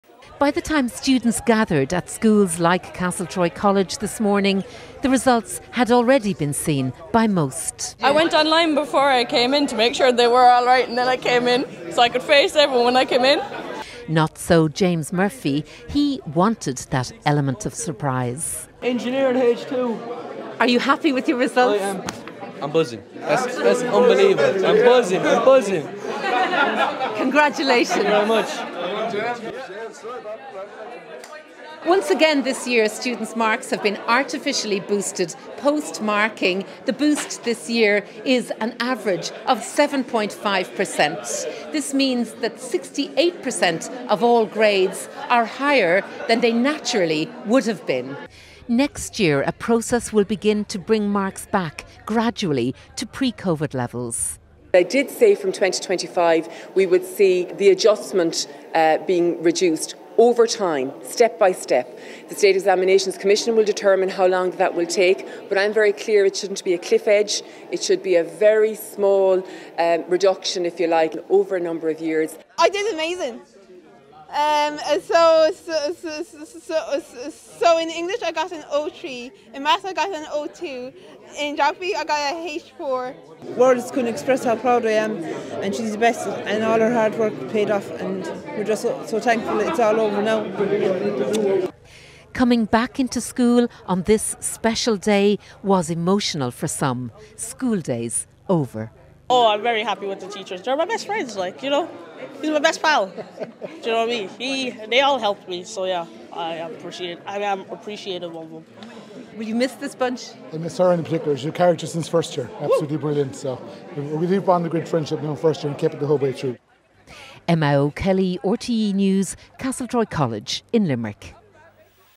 Leaving Cert exam inflation to be removed gradually
Minister for Education Norma Foley has indicated that grade inflation measures, introduced post pandemic, are set to be reduced from 2025 onwards on a "step-by-step basis".